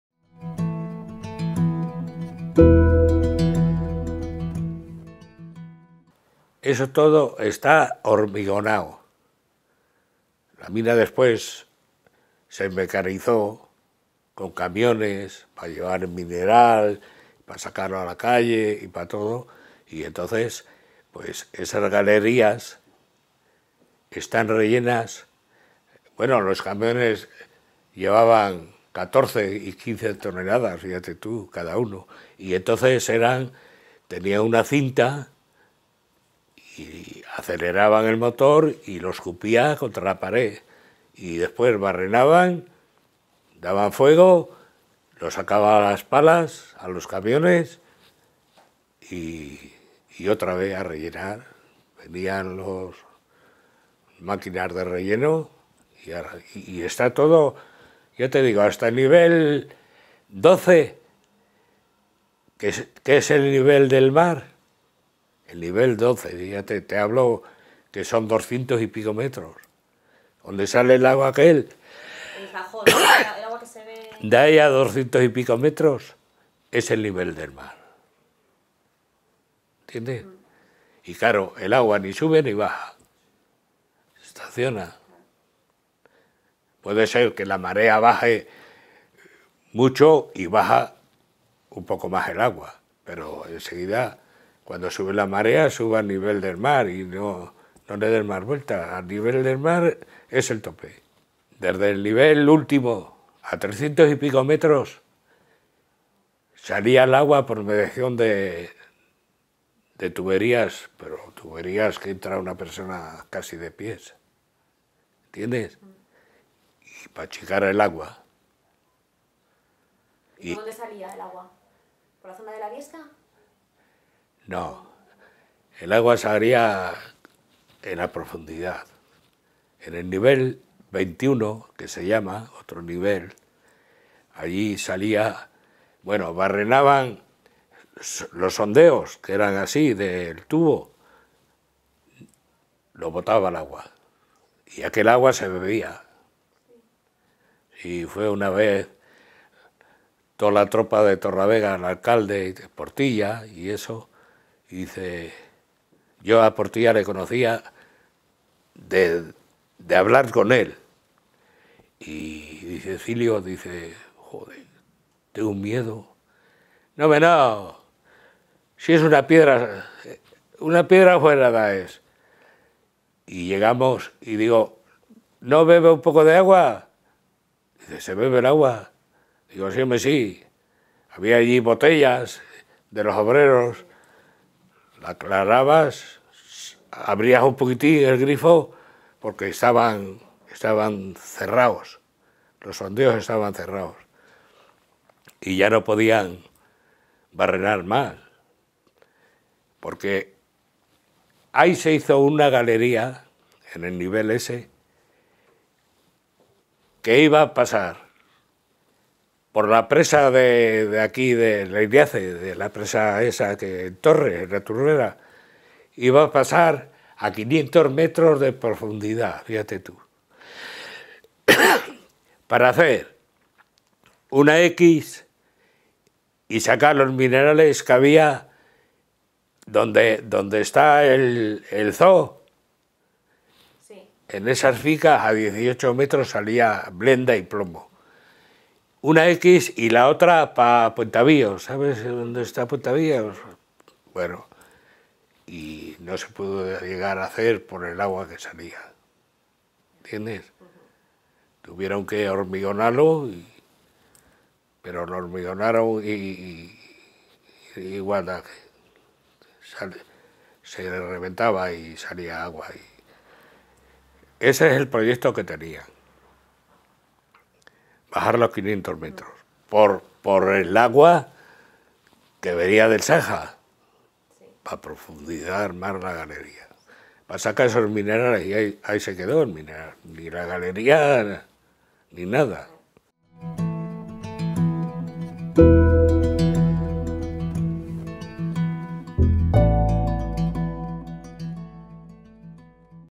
Sondeos en la Mina de Reocín
Cecilio Goicochea nos cuenta sus recuerdos como trabajador de la mina de Reocín. Cecilio recuerda varias anecdotas sobre los sondeos y como achicaban agua en la mina de Reocín.

En Primera Persona es un proyecto de conservación del patrimonio inmaterial de Cantabria a través de la tradición oral.

00´05´´ - 04´43´´ Cecilio explica cómo se hacían los sondeos y se barrenaba. Nos relata anécdotas curiosas sobre el agua que achicaban de los niveles inferiores de la mina de Reocín que se encuantran por debajo del nivel del mar.